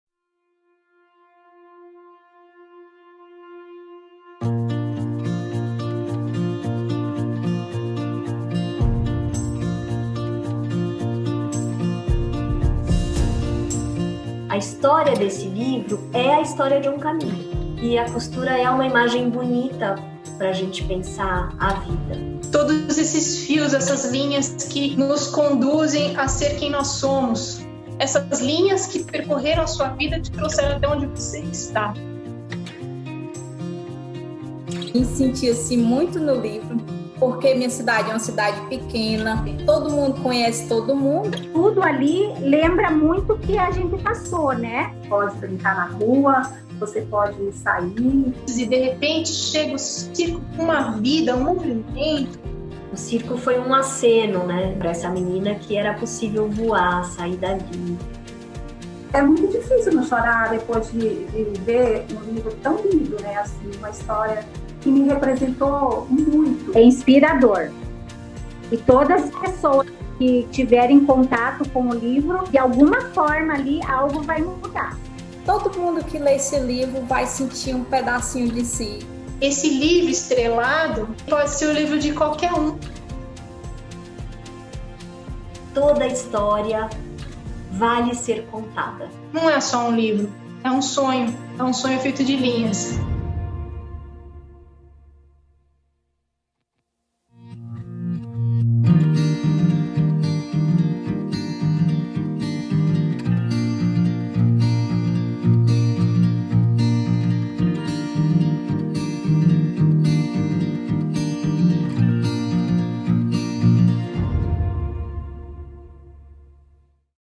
Costurando a narrativa: Um Sonho Feito de Linhas (teaser)
Das histórias de Consultoras de Beleza Natura de diferentes partes do Brasil nasceu um livro: “Um Sonho Feito de Linhas”, escrito por Ana Carolina Carvalho e ilustrado por Andreia Vieira. O livro faz parte de um movimento pela alfabetização de todas as crianças do Brasil até os 7 anos de idade.

O movimento EuEscrevoEssaHistória começou a ser construído no início do ano de 2020. Com a pandemia do novo coronavírus, entramos em isolamento social no dia 13 de março. A partir dessa data, todo o desenvolvimento e gravação dos conteúdos, inclusive audiovisuais e sonoros, respeitaram as normas de segurança para que nenhuma pessoa fosse colocada em risco. As gravações ocorreram em casa, sem aglomerações, apenas com orientações à distância da nossa equipe.